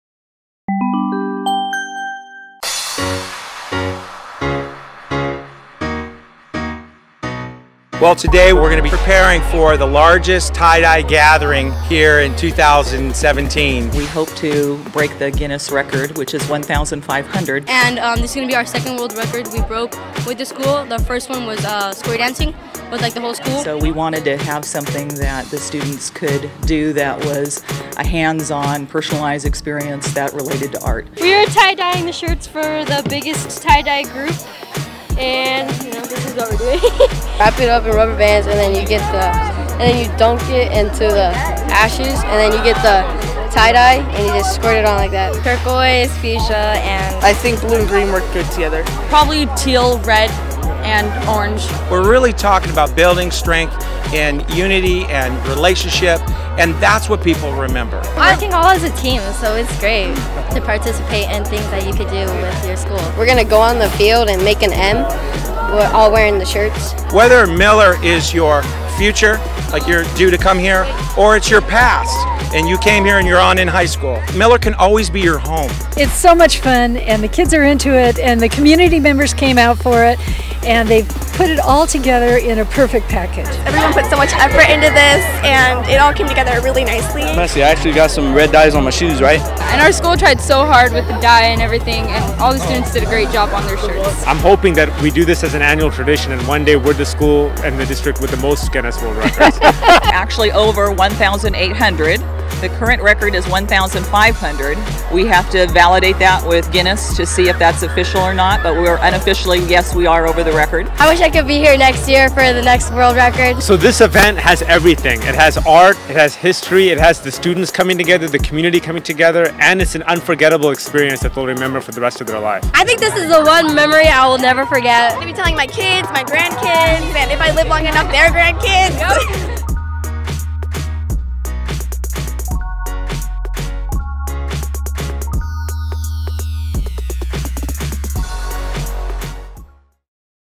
Miller Middle School Attempts 2nd Guinness World Record with Tie-Dye
Frank Augustus Miller Middle School has done it again! They have broken yet another Guinness World Record!! This time, it was for the largest gathering of people wearing Tie Dye Shirts. Technically, it still has to be verified by Guinness, but Miller’s numbers show they exceeded the previous world record by a whopping 20%. Congratulations to the students, who made their own shirts, the staff, all the other schools, officials and community members who came out to help Miller take this commanding lead in the race to bring back 1960's fashion!

Cinematography: Marc Galang & Khanh Pham
Editor: Marc Galang